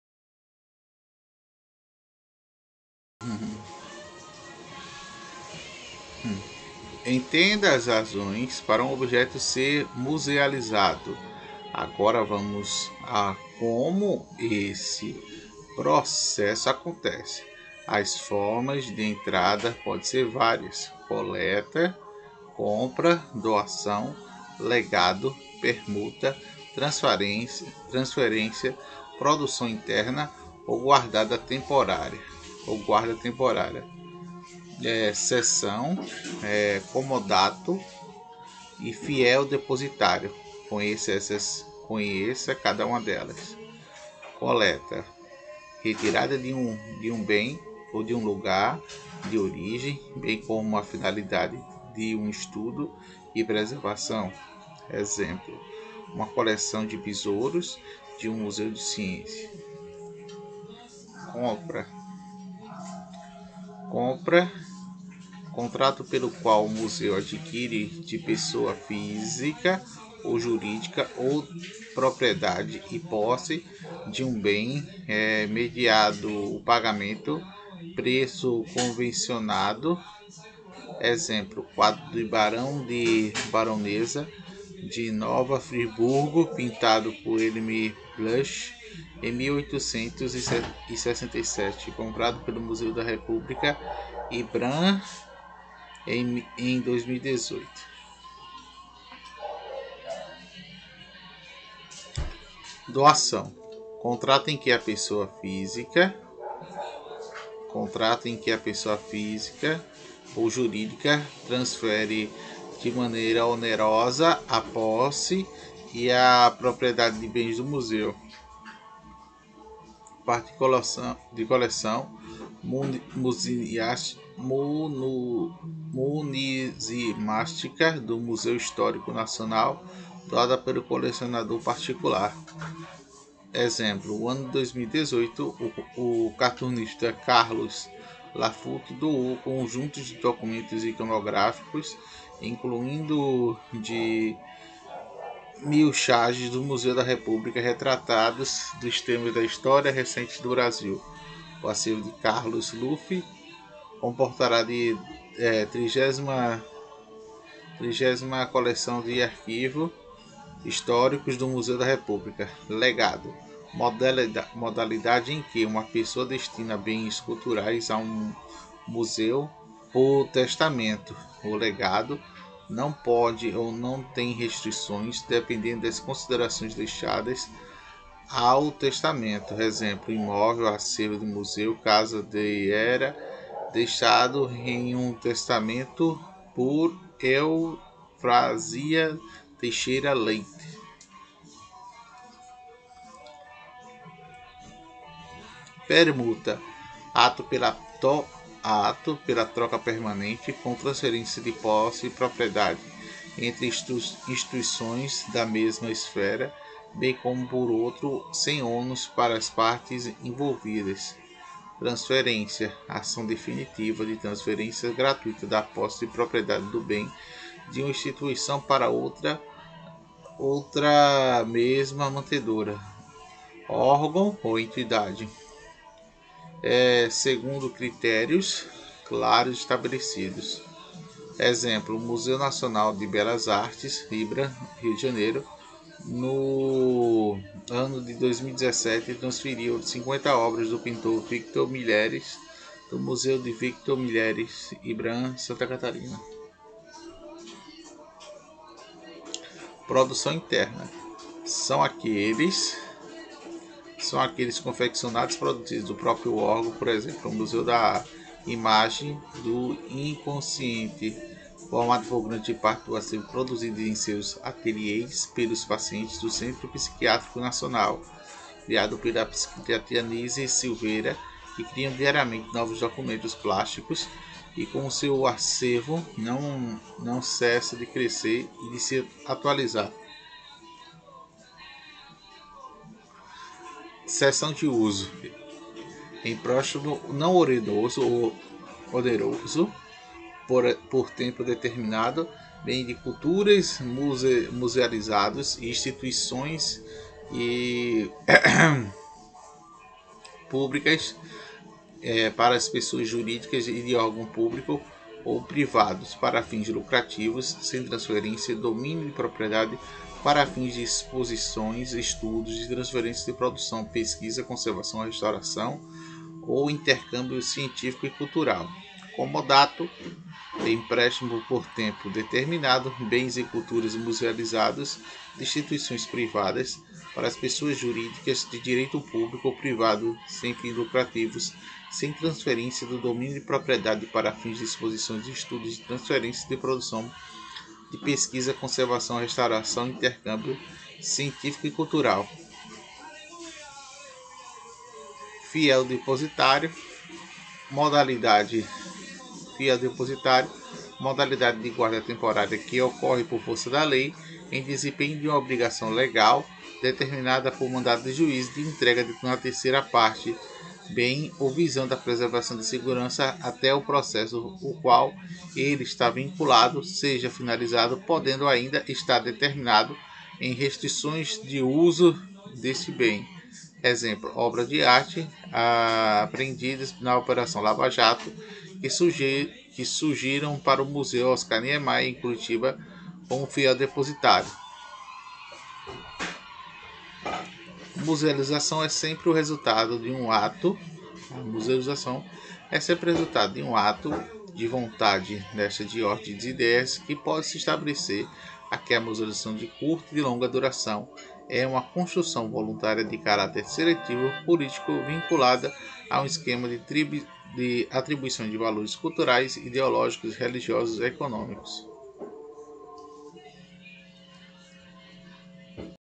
Explorando Formas Musealizadas de Entrada
Entendidas as razões para um objeto ser musealizado, agora vamos ao “como” esse
processo acontece. As formas de entrada podem ser várias: coleta, compra, doação,
legado, permuta, transferência, produção interna ou guarda temporária (cessão, comodato e fiel depositário). Conheça cada uma delas: